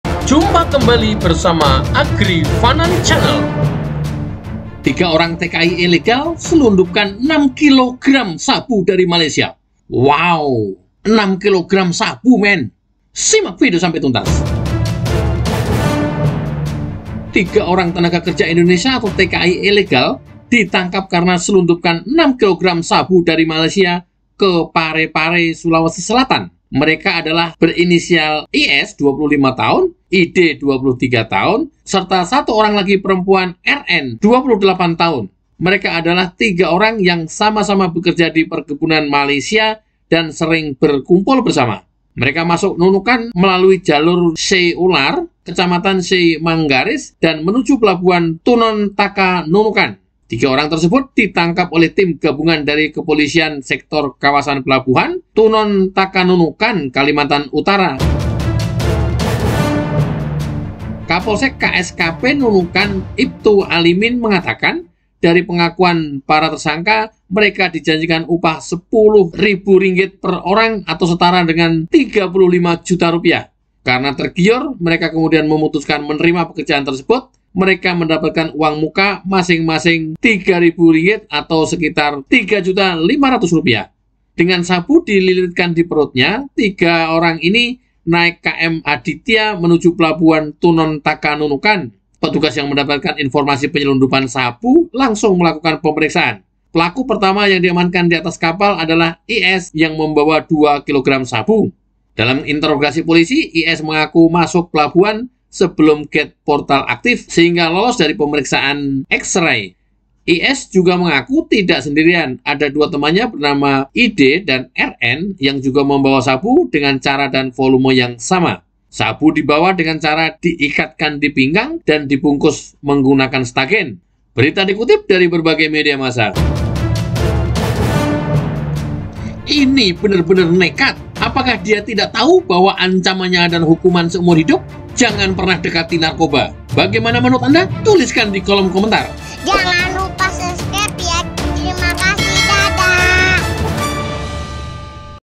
BENAR-BENAR NEKAD, TKI PULANG KE INDONESIA BAWA OLEH-OLEH S4BU SEBERAT 6 KG
Tiga tenaga kerja Indonesia (TKI) ilegal ditangkap karena selundupkan 6 kilogram sabu dari Malaysia ke Parepare, Sulawesi Selatan. Mereka ada IS (25) dan ID (23), serta seorang perempuan RN (28). Mereka adalah tiga orang yang sama-sama bekerja di perkebunan Malaysia dan sering berkumpul bersama. Tiga orang tersebut ditangkap di Nunukan, Kalimantan Utara, pada Rabu (16/2/2022) sekitar pukul 11.00 Wita.